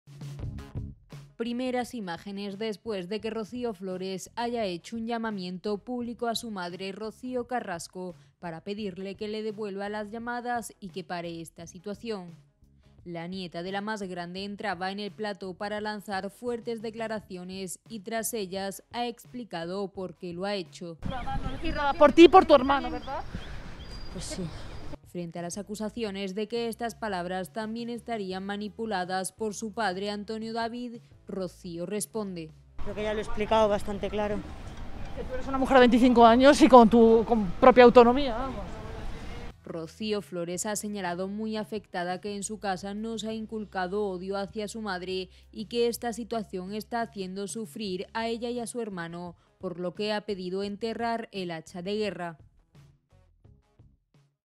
Primeras imágenes de Rocío Flores tras el llamamiento a su madre
Primeras imágenes después de que Rocío Flores haya hecho un llamamiento público a su madre, Rocío Carrasco, para pedirle que le devuelva las llamadas y que pare esta situación. La colaboradora entraba en el plató para lanzar fuertes declaraciones y tras ellas, ha explicado por qué lo ha hecho.